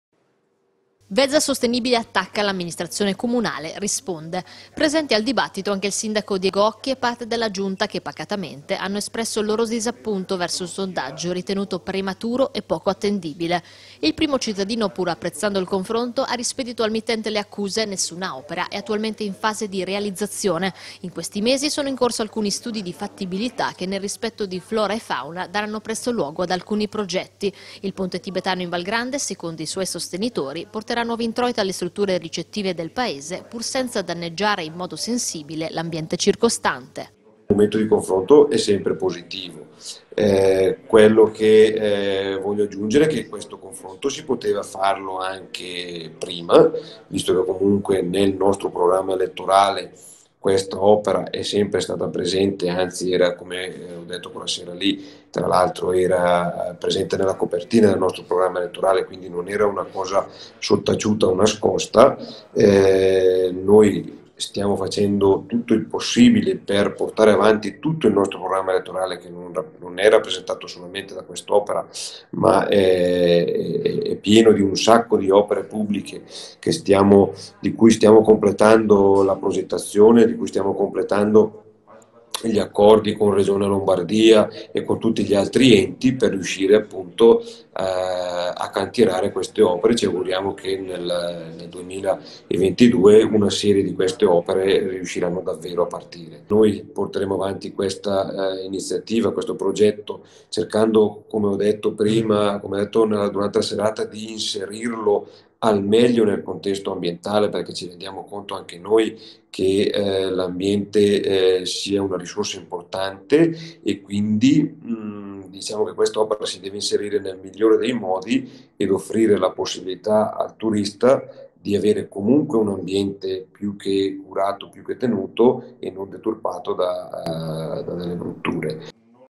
UN’ATTRAZIONE RISPETTOSA DELL’AMBIENTE
GRAZIE!

Basso impatto ambientale e più introiti per le strutture ricettive. L’amministrazione comunale di Vezza d’Oglio risponde alle accuse e rilancia l’idea del ponte in Val Grande.